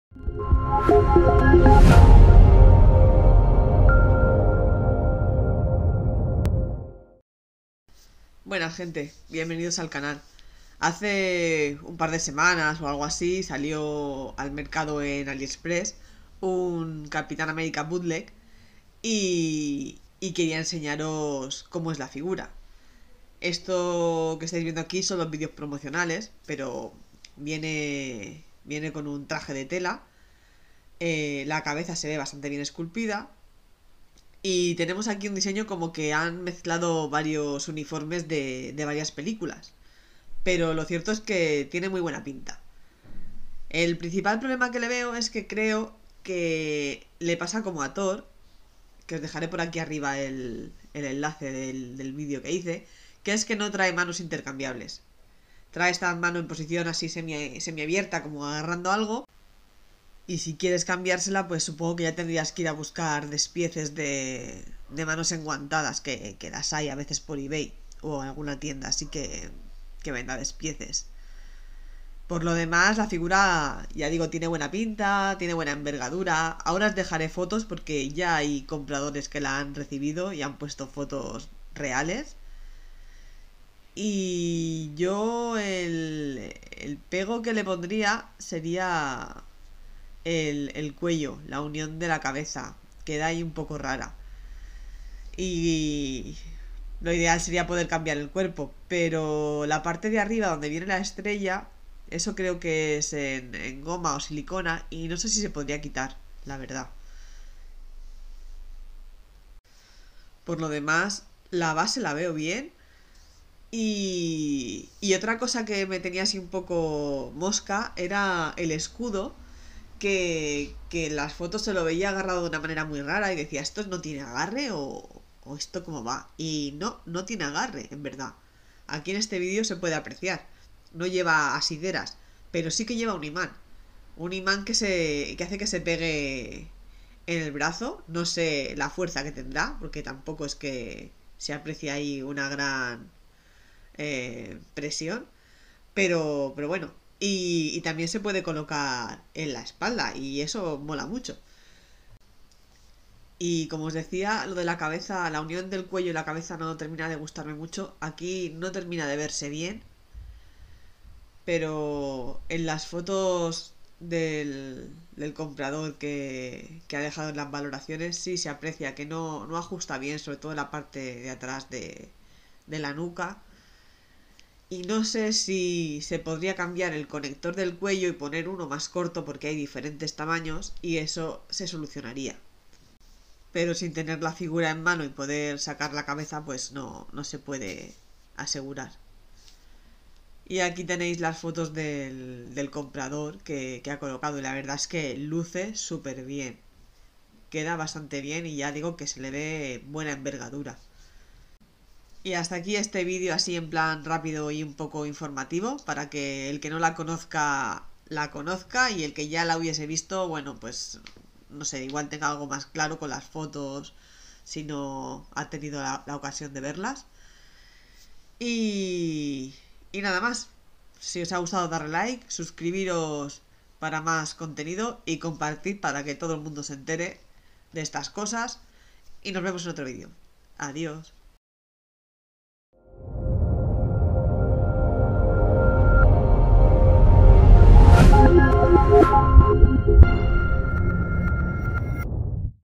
Capitan America 1/6 LOW COST Aliexpress
En este vídeo os dejo una primera muestra de lo que es la nueva figura low cost de Capitán América que se puede conseguir en Aliexpress, la marca no la tengo clara, en algunos anuncios dicen que son Crazy Toys (Juguetes locos) pero en la caja no aparece. Son unos vídeos promocionales de la figura y unas fotos de compradores reales.

Capitan america
- Battle Damage

La mejor manera de apoyarme, es dando LIKE y COMPARTIENDO.

También podéis hacer una pequeña donación